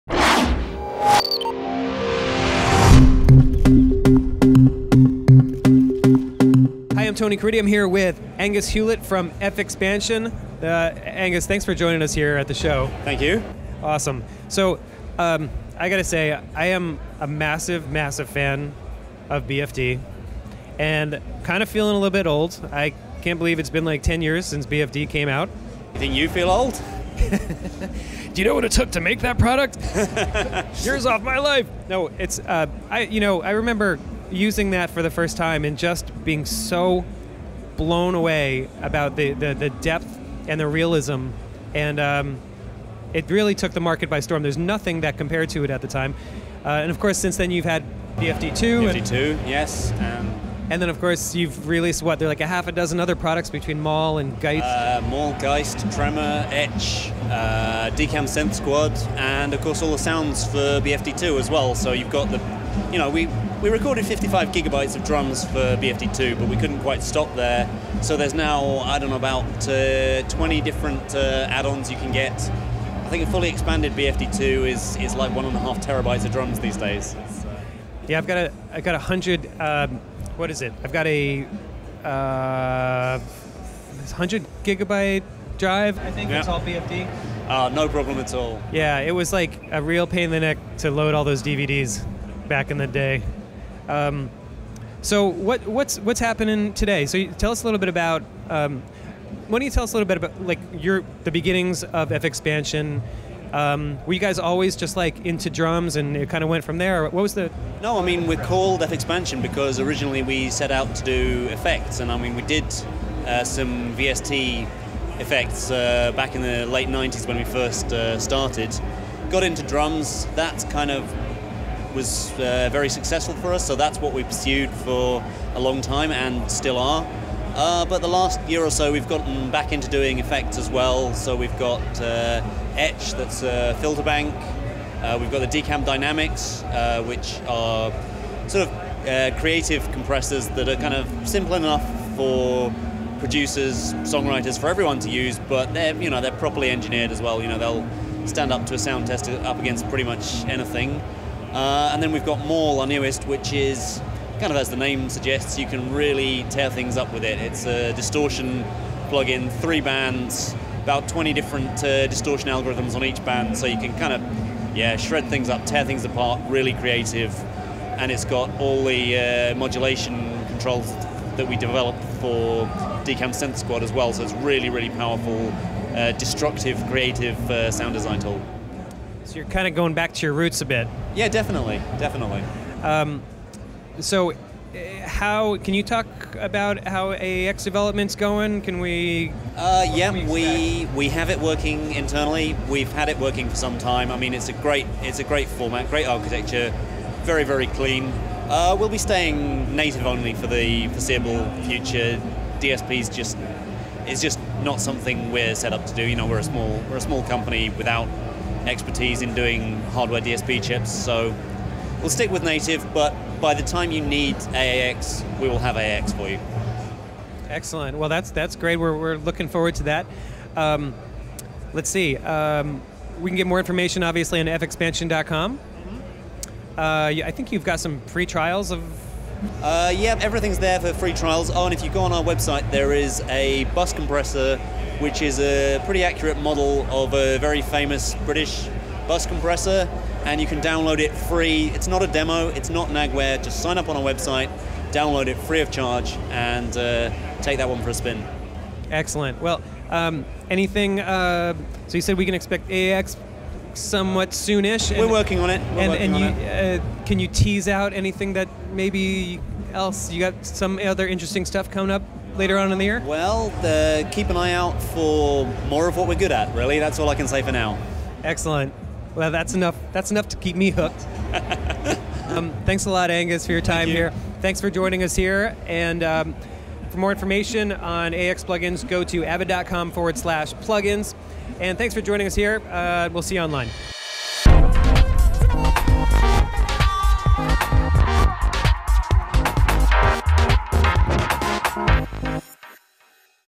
NAMM 2013: AAX Plug-in Developer FXpansion
FXpansion has established a growing range of plug-ins that inspire a large and devoted userbase including BFD2, a premium production environment for acoustic drums; GEIST, a sampling drum machine with advanced ergonomics; DCAM: SYNTH SQUAD, an analogue-modelled synthesis suite; and TREMOR, a forward-thinking drum synthesizer.